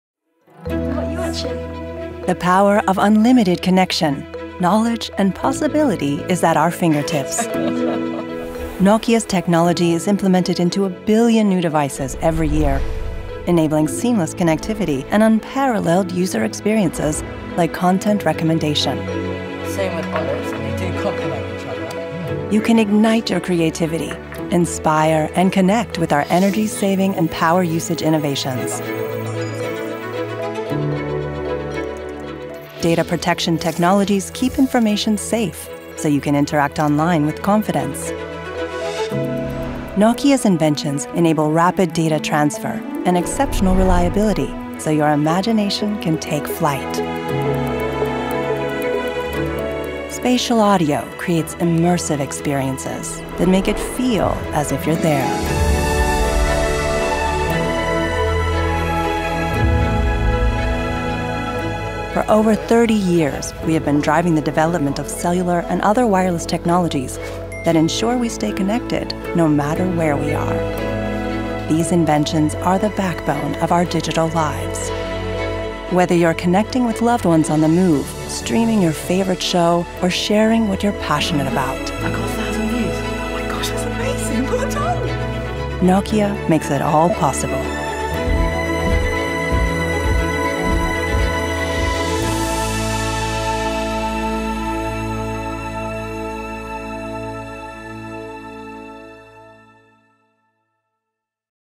How Nokia keeps billions of devices connected
Discover how Nokia powers seamless connection, creativity and security in a billion new devices every year.

With over three decades of relentless innovation and 20 000+ patents, Nokia has defined the core technologies and open standards that power billions of devices worldwide. Today, our inventions underpin entire industries—from smartphones, consumer electronics, and video/multimedia services to automotive connectivity and the broader IoT ecosystem.